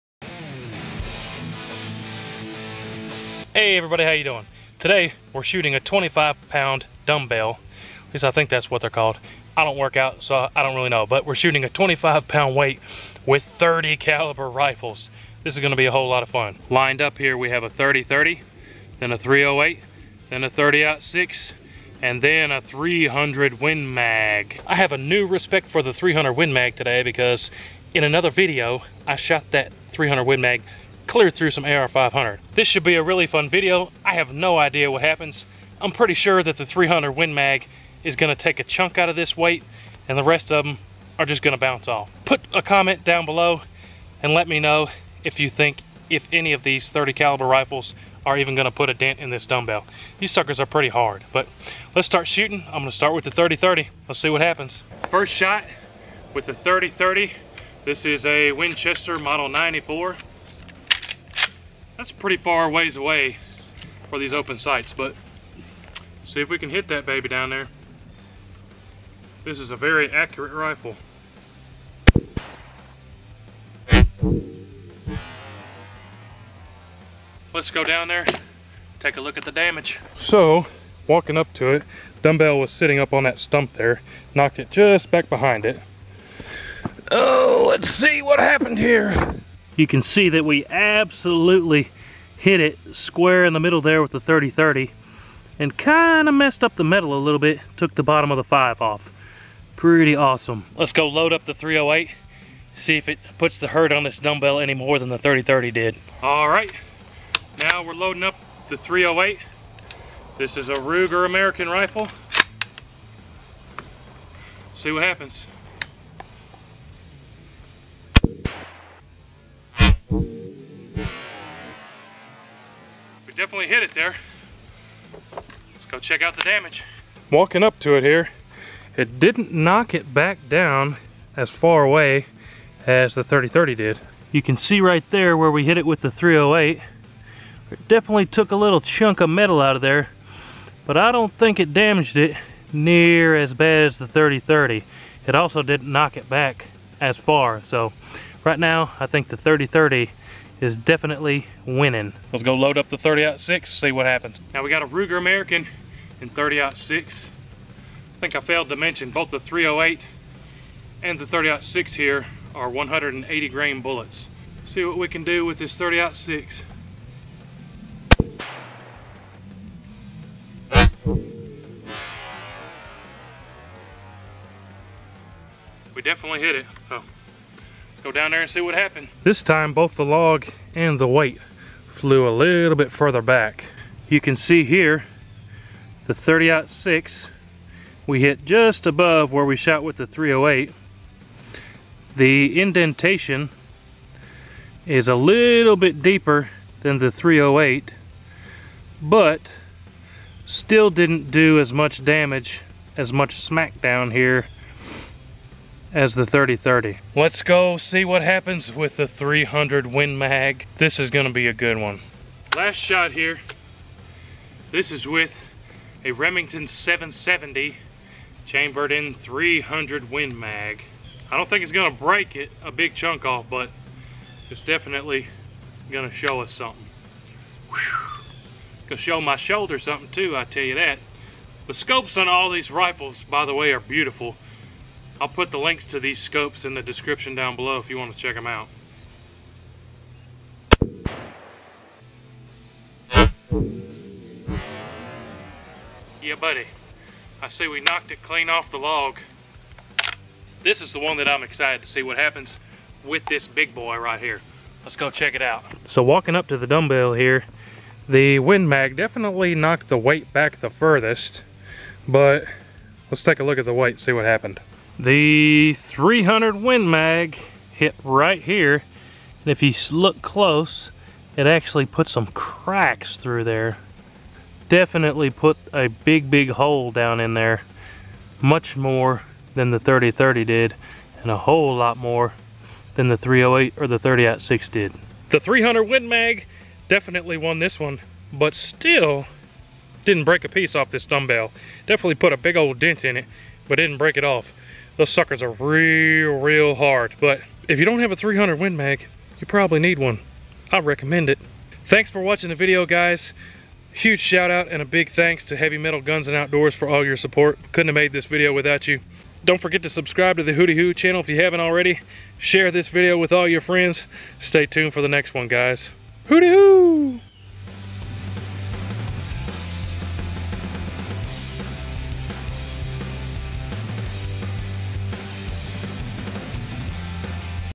Shooting a Dumbbell - 30-30 vs .308 vs 30-06 vs 300 win mag
Impact ear protection